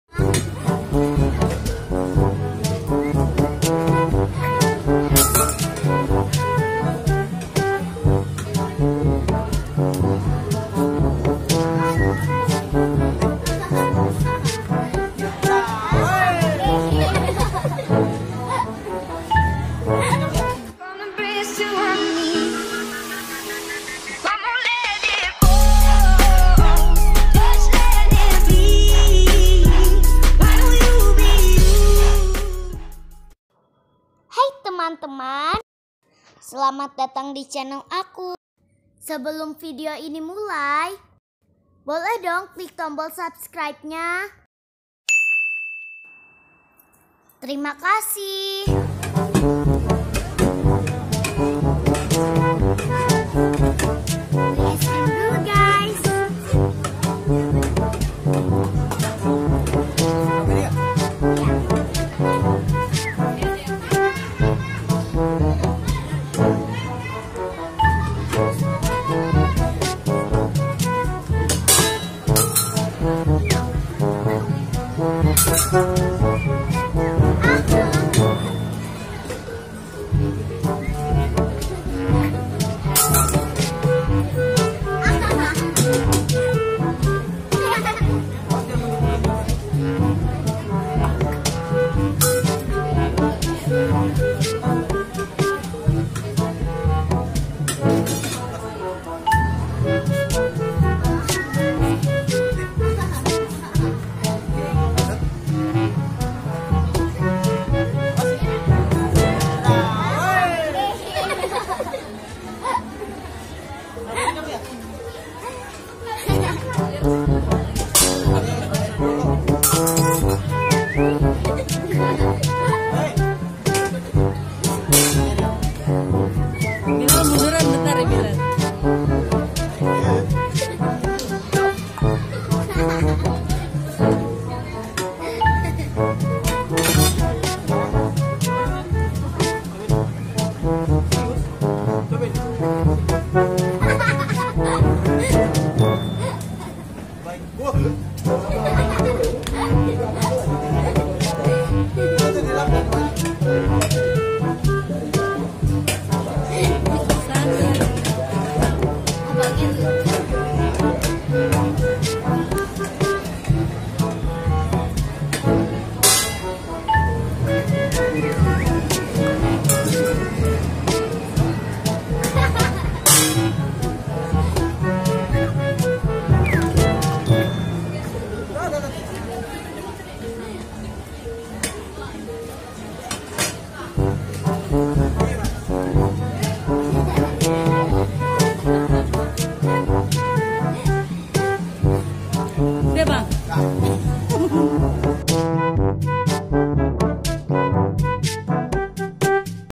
Atraksi penjual ice cream cone | auto ngakak
Teman2 lucu deh penjual ice cream cone atraksi nya. Penasaran kan lngsung tonton aja ya..
tonton video Aku yang lain nya juga ya

Selamat menonton

Terima kasih